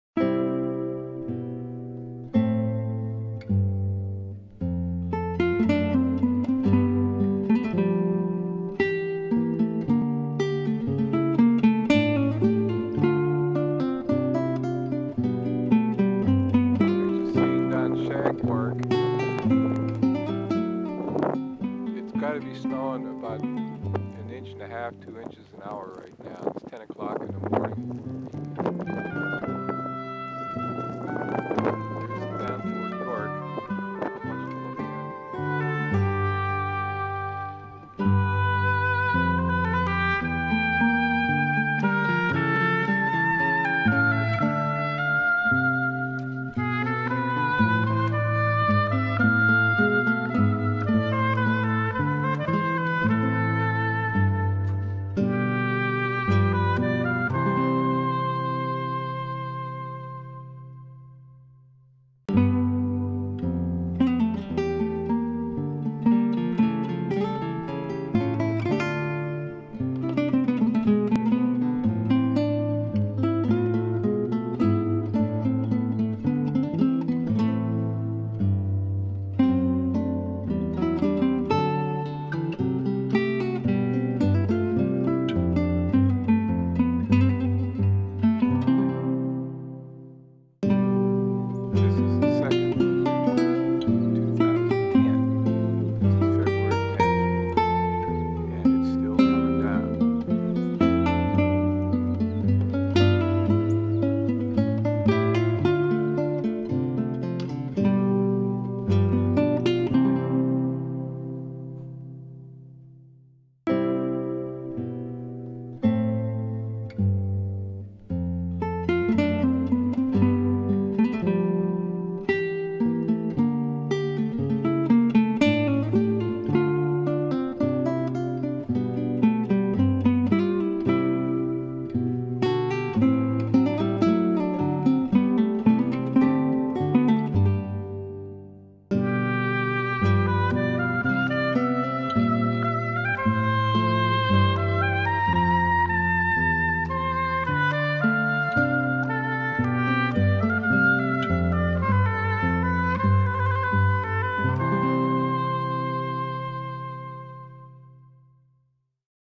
Snow at White Oak Manor
This shows pictures of the February 2010 snow storm at White Oak Manor in York, PA.